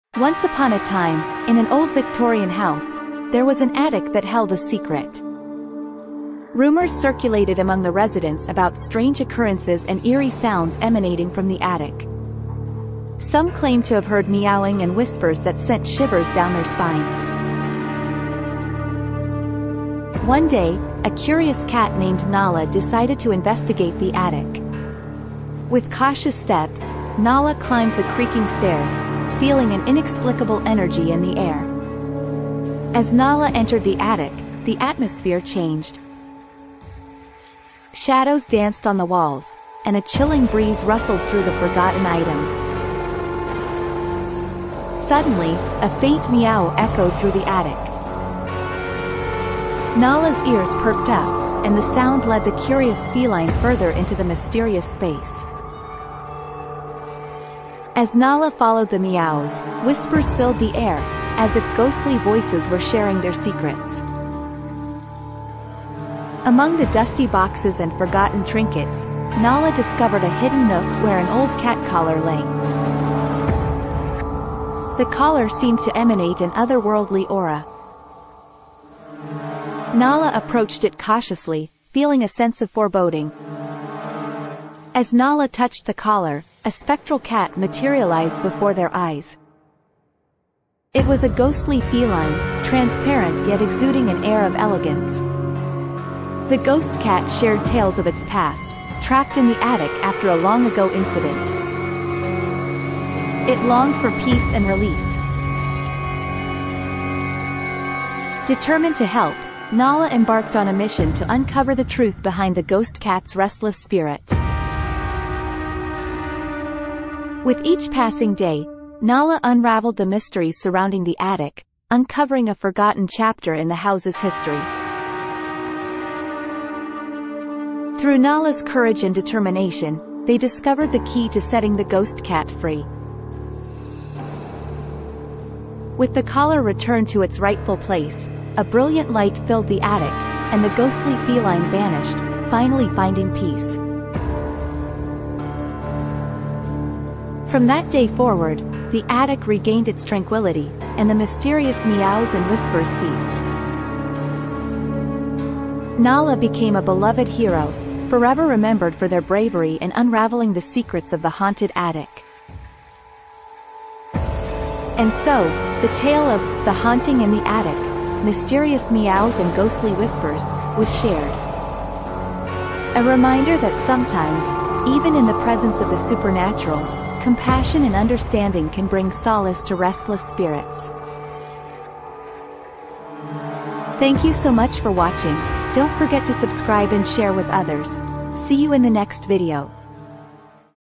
Cursed Collar Chronicles: Nala's Quest to Set the Ghost Cat Free
Experience the captivating tale of a ghostly cat trapped in a haunted attic. Follow Nala, a curious feline, as she unravels the mysteries of the ghost cat's past and embarks on a journey to bring peace and release to the restless spirit. This heartwarming story of love, loss, and redemption will enchant you. Discover the secrets of the attic in this hauntingly beautiful narrative.